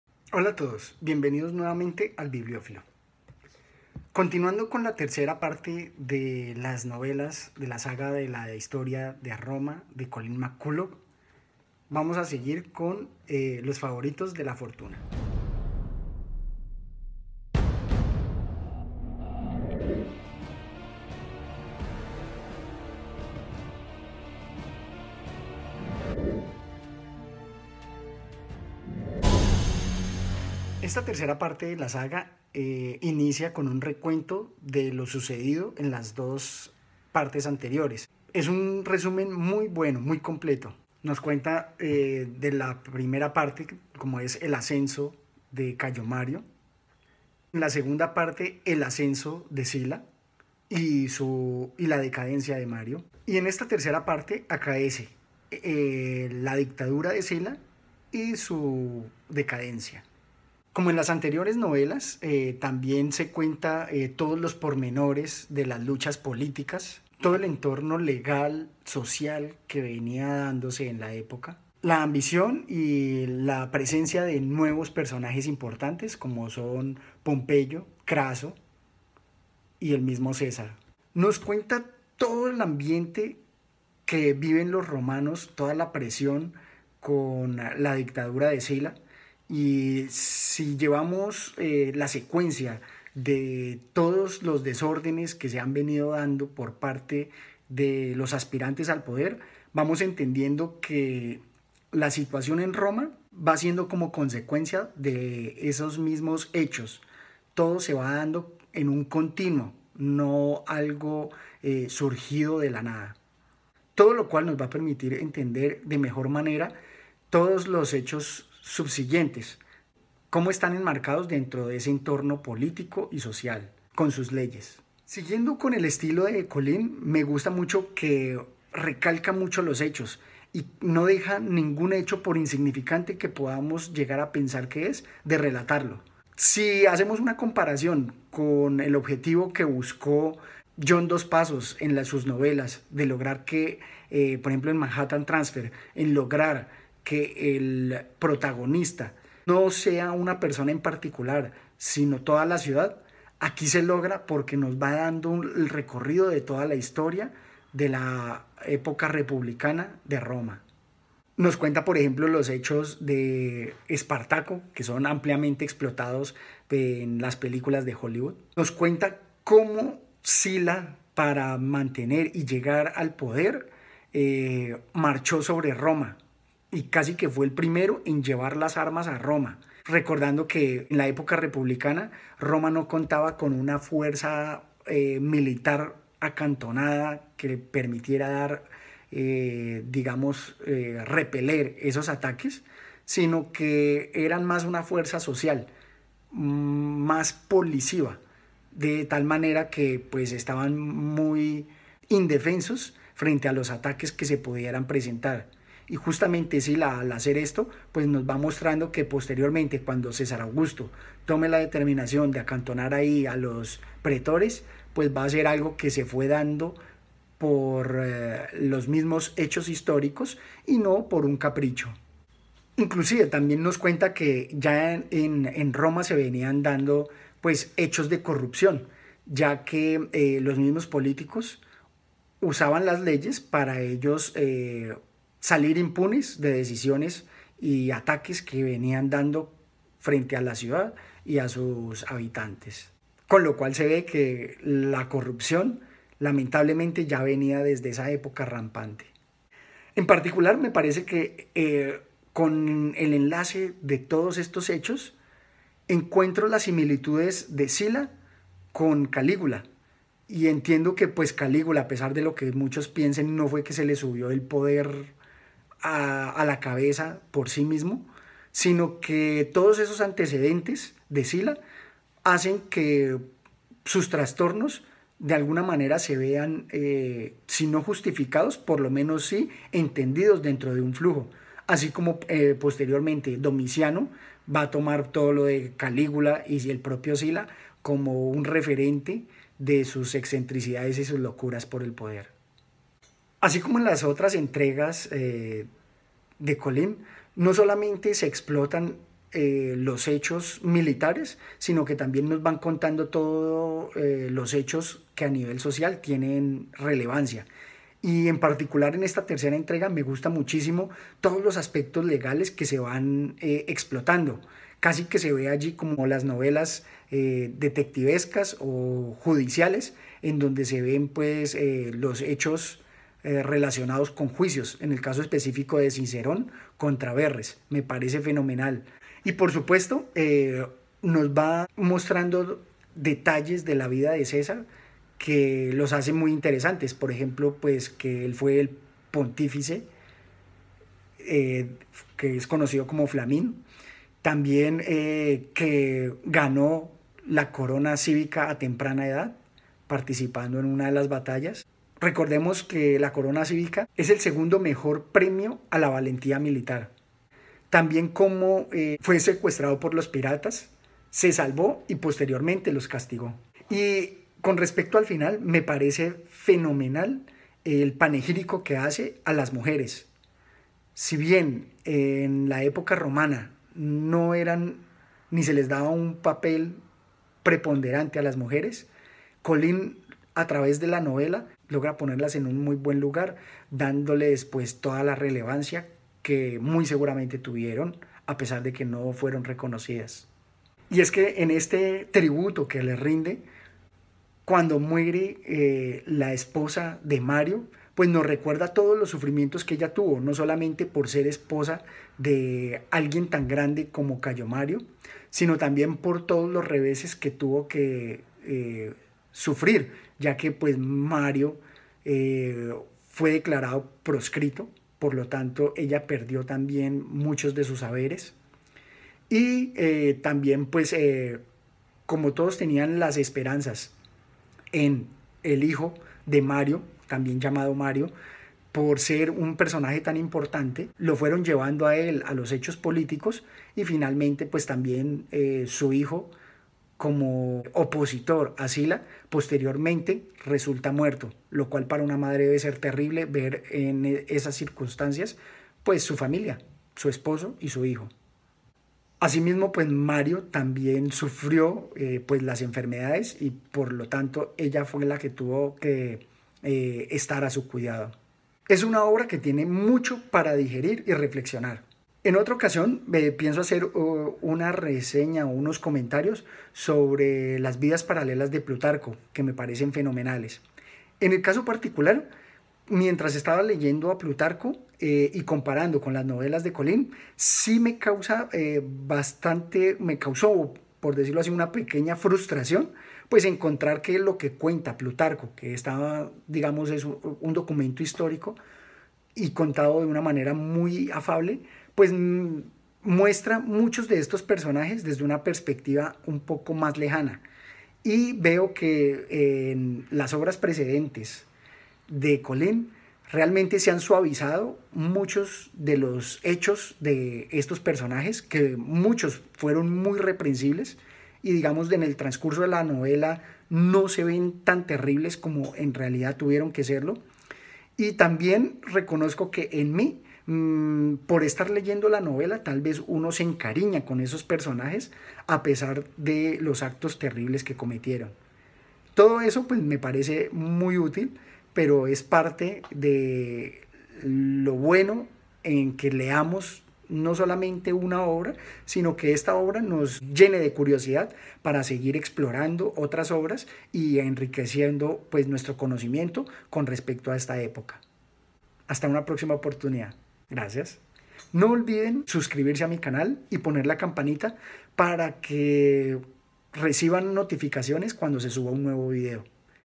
Favoritos de la Fortuna - Colleen McCullough
Tercera parte de la saga de la antigua Roma. 1993